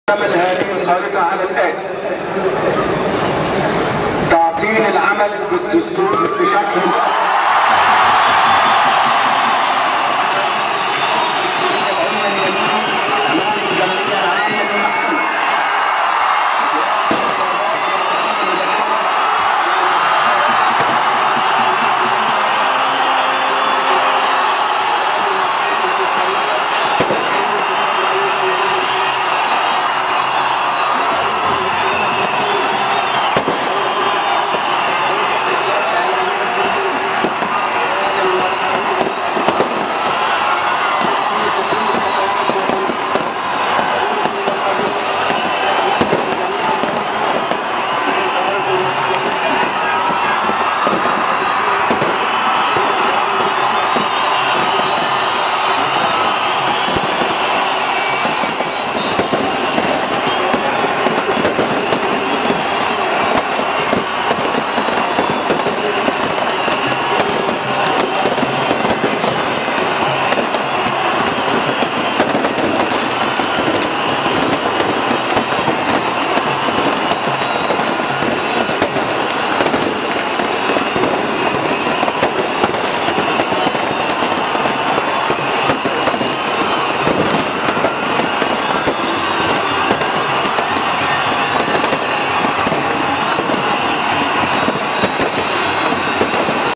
Egypt army announces ouster of president Mohamed Morsi - Presidential palace
Presidential Palace, Heliopolis, Cairo

After days of nationwide protests, Wednesday night the Egyptian Armed Forces announced the ouster of Muslim Brotherhood leader Mohamed Morsi, a new constitution, new elections and an interim government.

The crowds went wild.